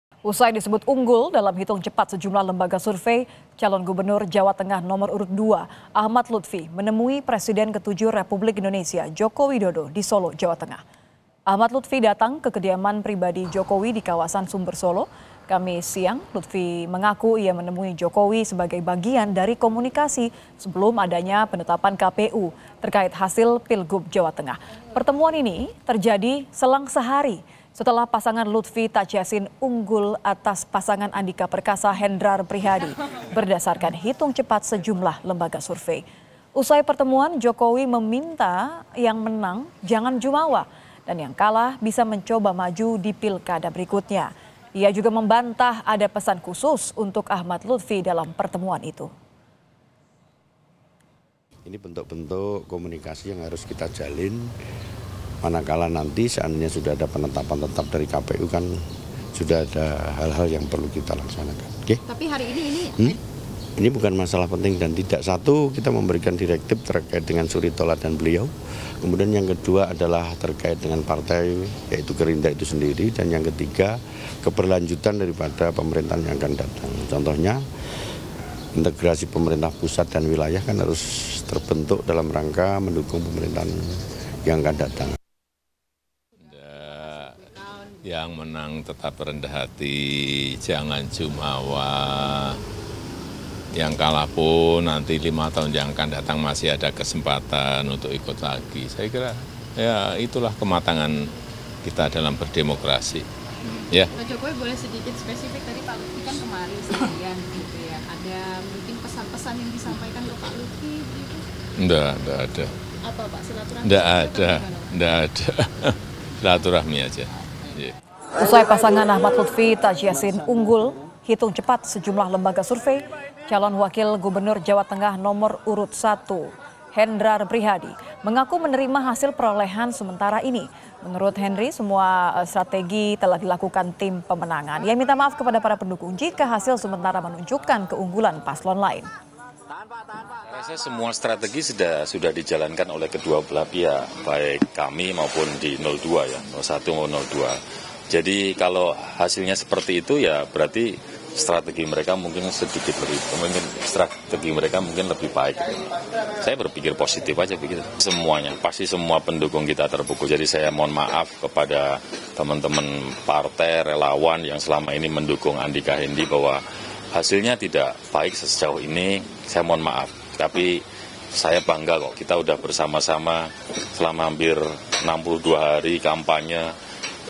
Ahmad Luthfi Temui Jokowi di Solo Usai Unggul di Hitung Cepat
KOMPAS.TV - Usai Unggul di Hitung Cepat Cagub Jateng Ahmad Luthfi Temui Jokowi di Solo